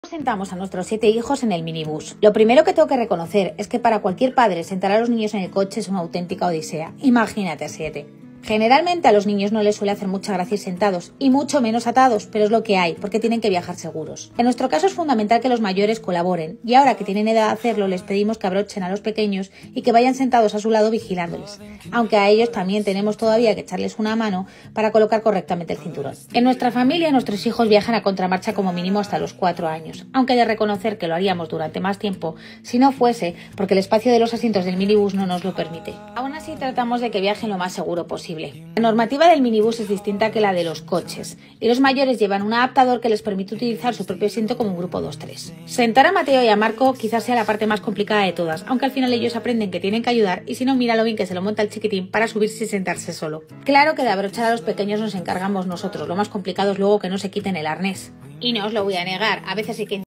Como sentamos a nuestros 7 hijos en el minibús
Cómo nos organizamos para sentar a nuestrs 7 hijos en nuestro minbús